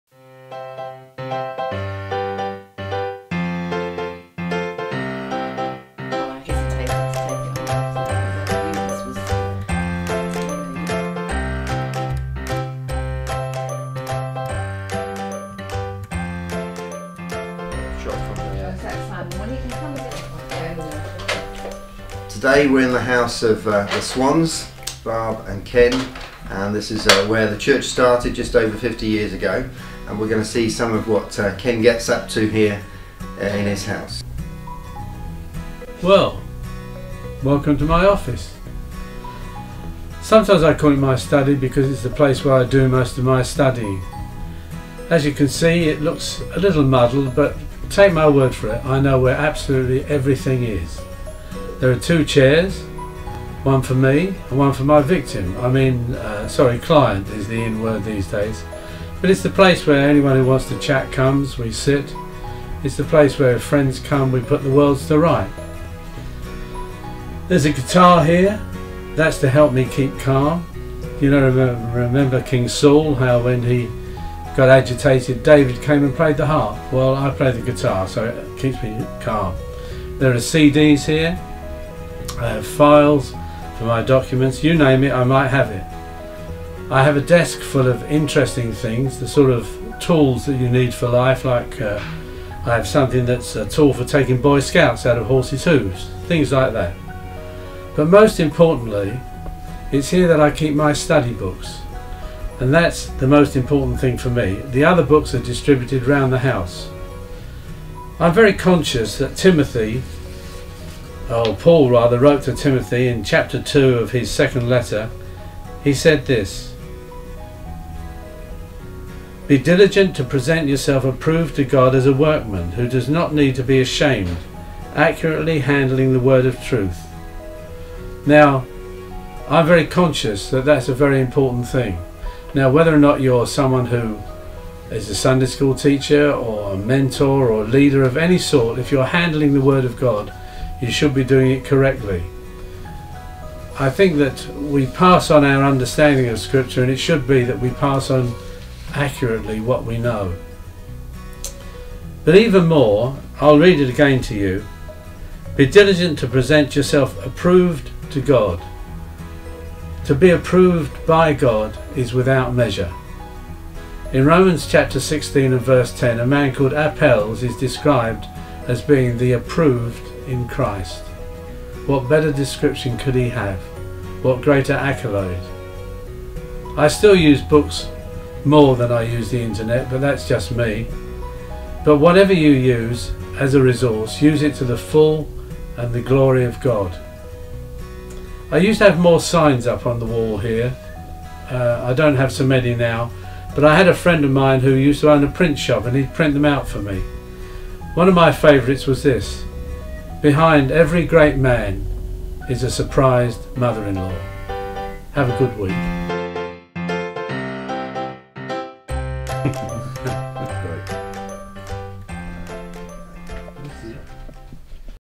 CFC vlog, 17/3
So here is the latest vlog update, this week we have gone beyond the four walls of Chase Family Church centre to the home of Ken and Barbara Swan.